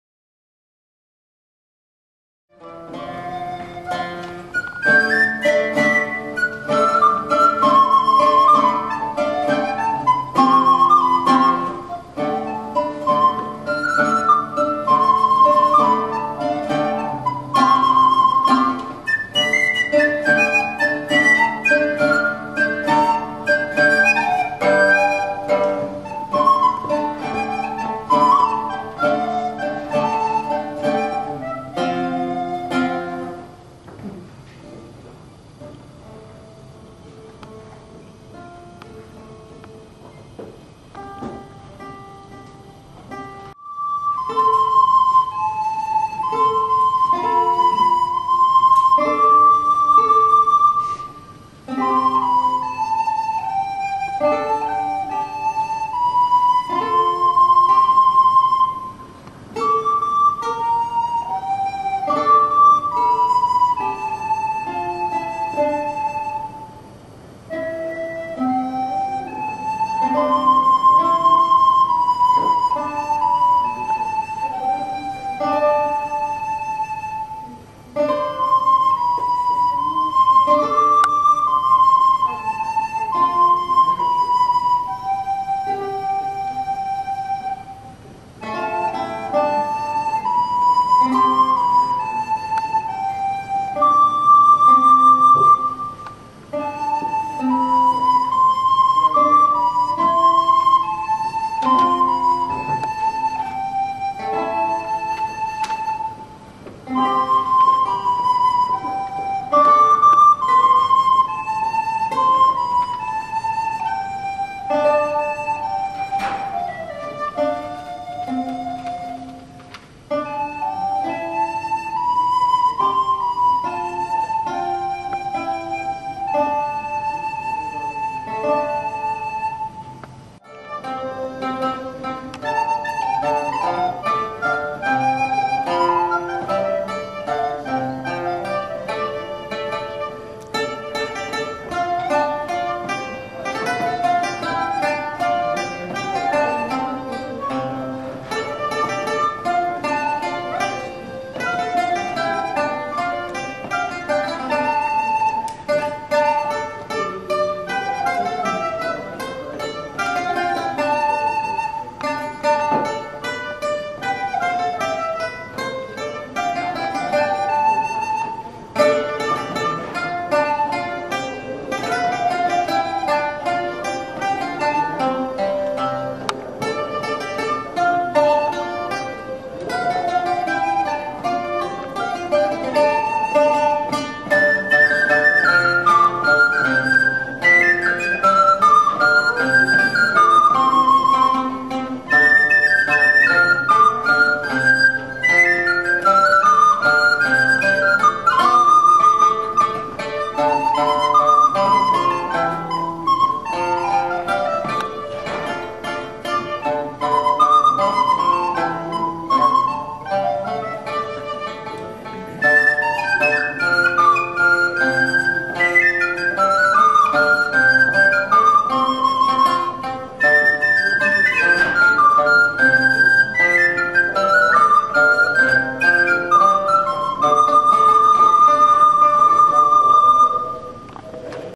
Duo Nova Antigua
Mary Cherney and Dennis Cinelli played a concert of old European music, on lute, mandolin, recorder, old flute and guitar, Dec. 6, 2008, Morris County Library (NJ).